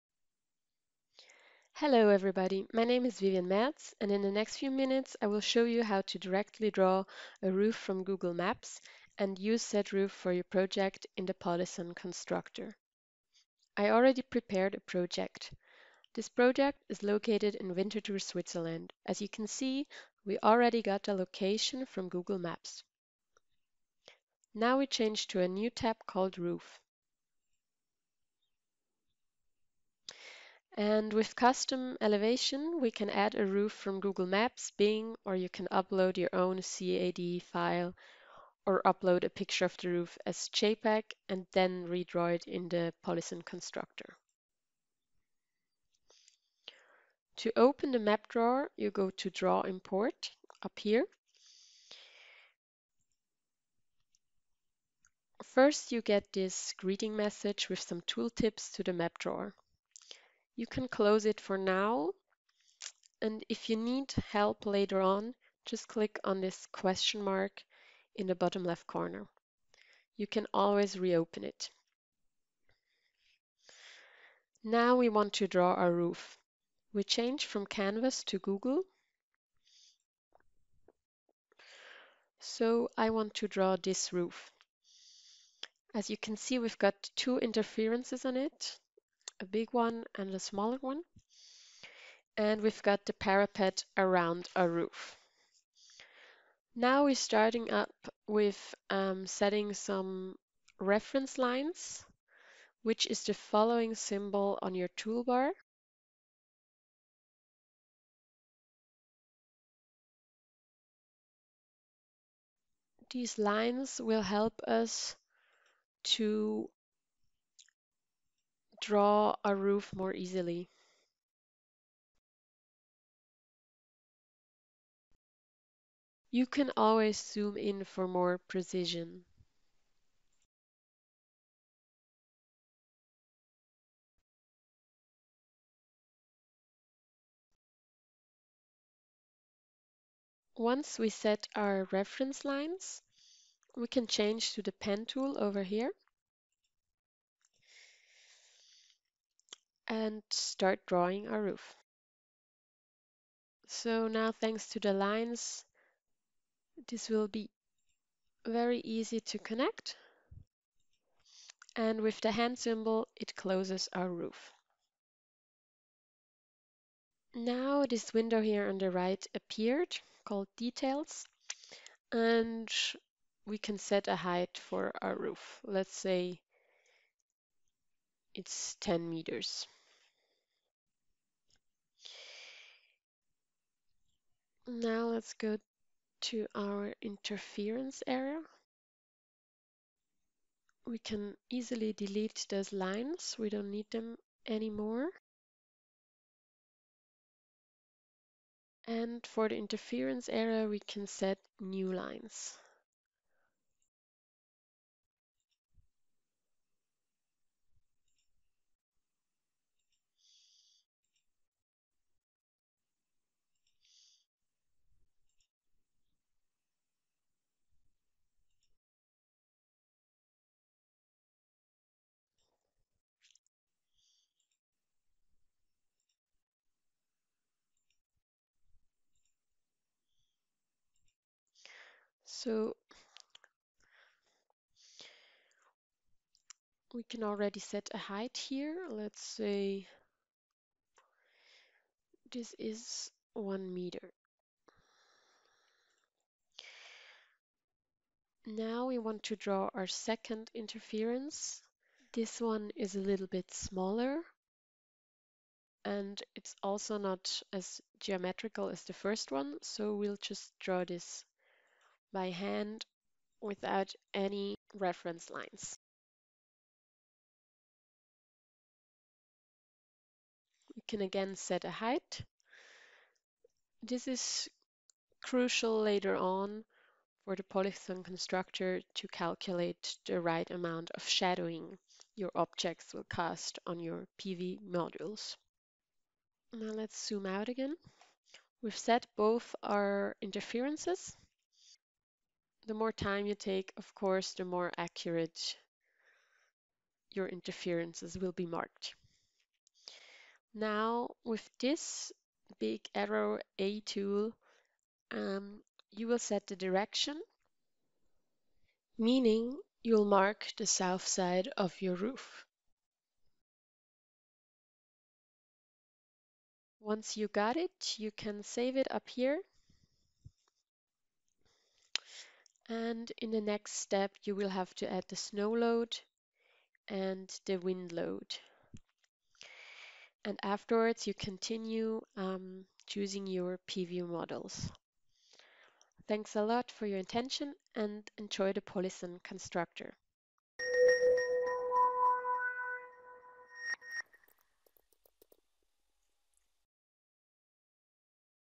Polysun Constructor Roof Planner (Google Maps)
In this short video we will demonstrate how you can plan your roof (incl. interferences) in the polysun constructor by making use of google maps.